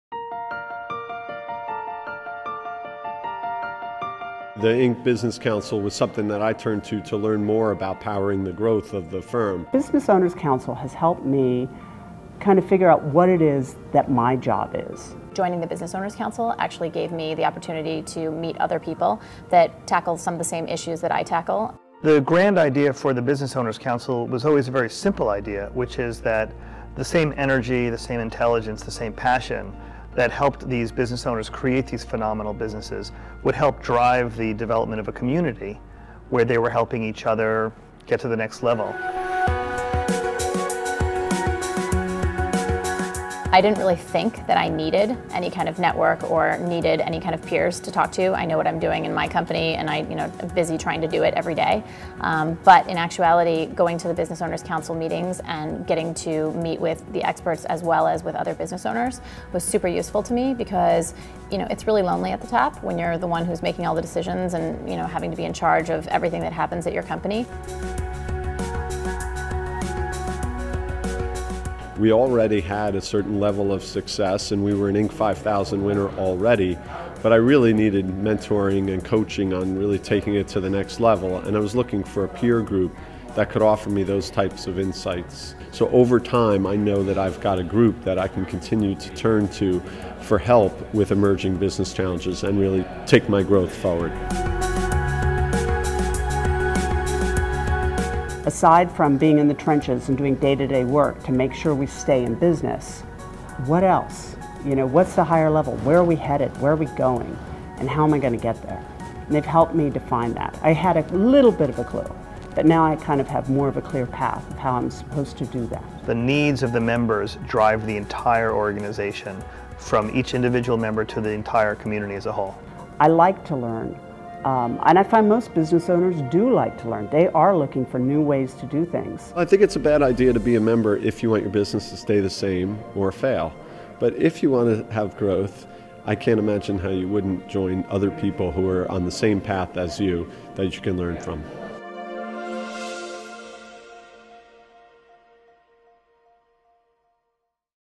Inc. Business Owners Council - Video Testimonials from Council Members
Inc. Magazine's Business Owners Council is a network of top performing entrepreneur in regional groups coast-to-coast. Through peer-to-peer events and online community, members of The Council meet, collaborate and advise each other.